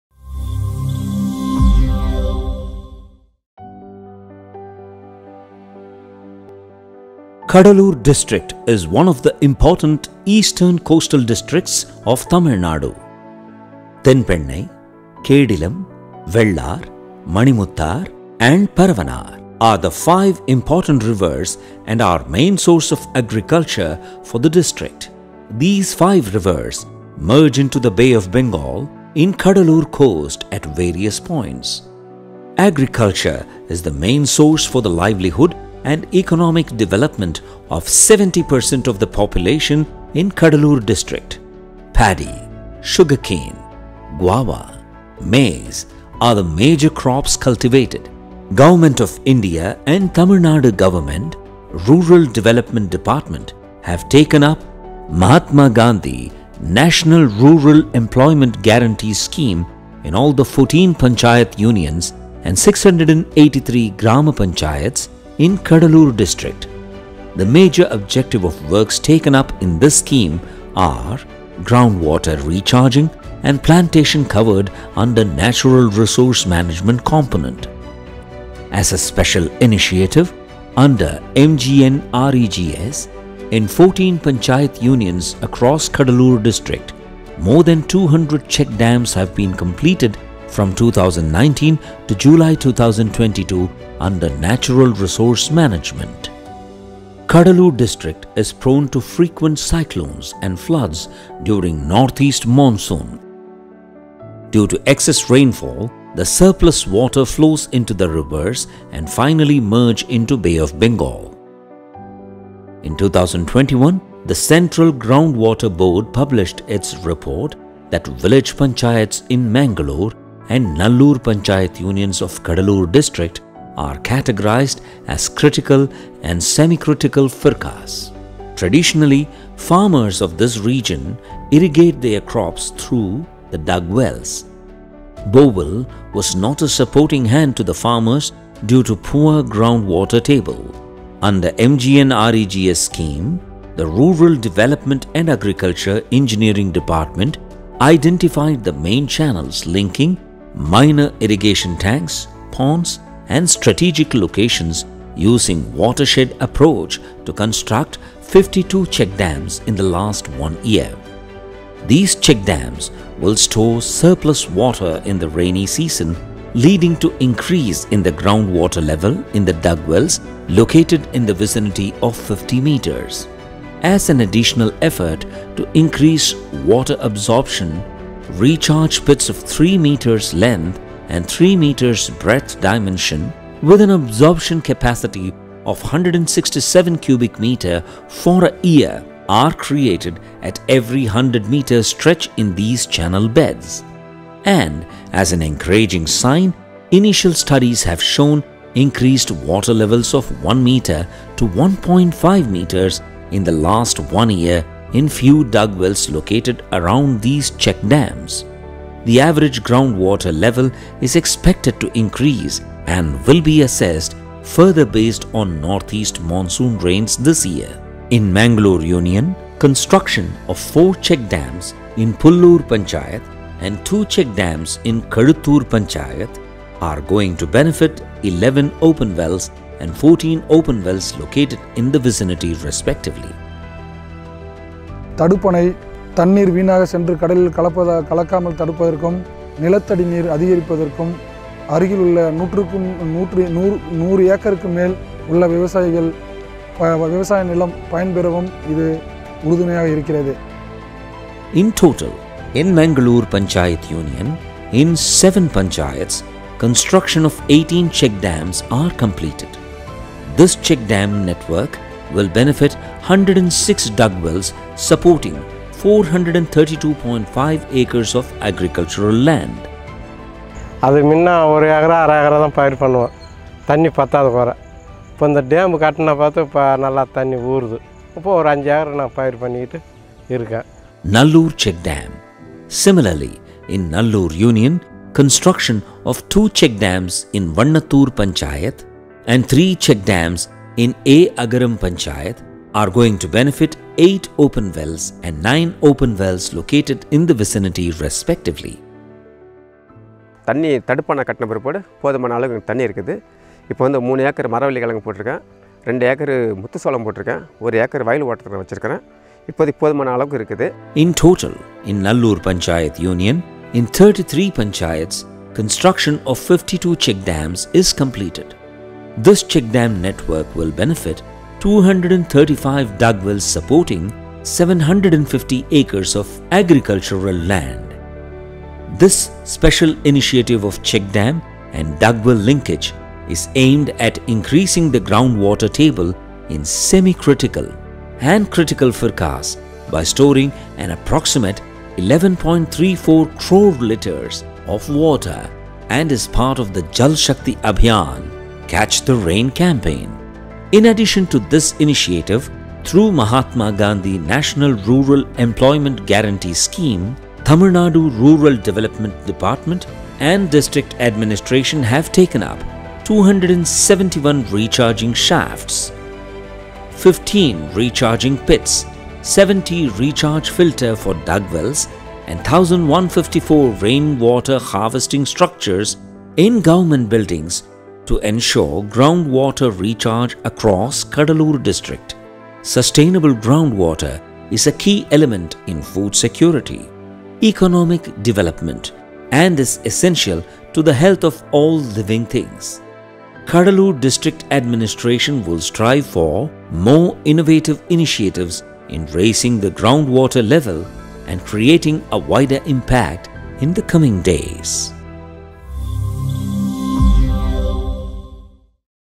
Case Study Video : Check Dams - Cuddalore District Administration - DRDA -TN Govt. - Eng
We are collaborating with the DRDA - cuddalore District Administration to create Project Case Study Videos for checkdam Projects being developed throughout the district. This Ground Water Recharge Level scheme was developed by
DRDA
Cuddalore District Administration
Tamilnadu Govt
MGNREGA
Mahatma Gandhi National Rural Employment Guarantee
Central Ground Water Board

Corporate Office:
Easton Media Pvt Ltd,
TS 70, 1st Floor, Guindy TVK Industrial Estate,
Ekkaduthangal, Chennai - 600032,
TN - India.